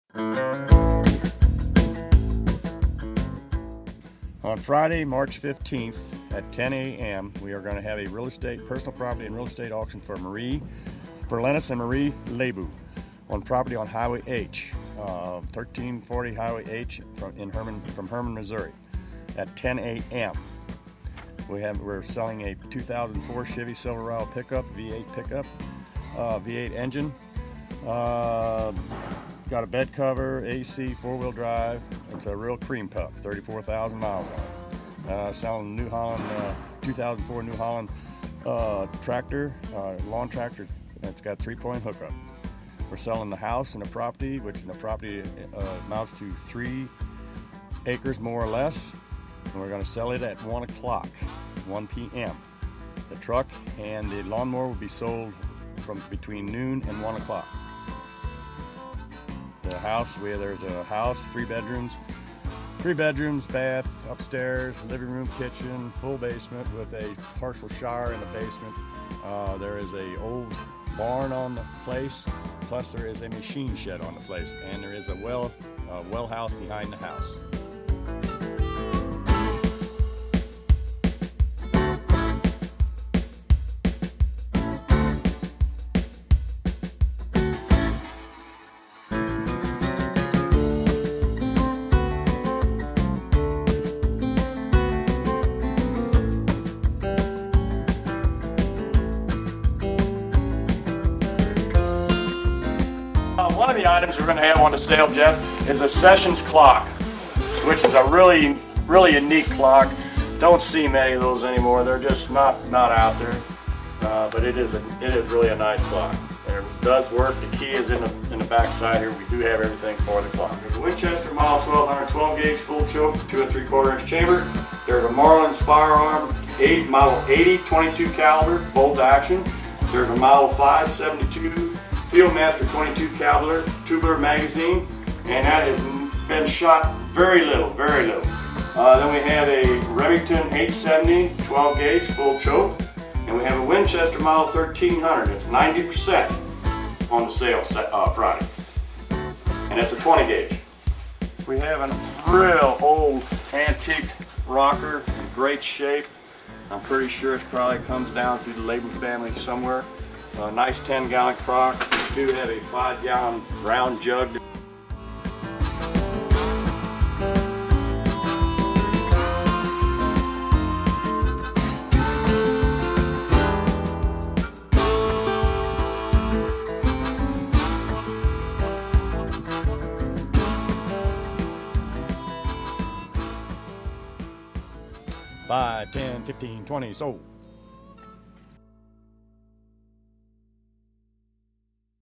Wehrle Auction MAR 15 2013
Beautiful 1960s home, 3 acres, a gorgeous pick-up truck, gorgeous lawn tractor, nice antiques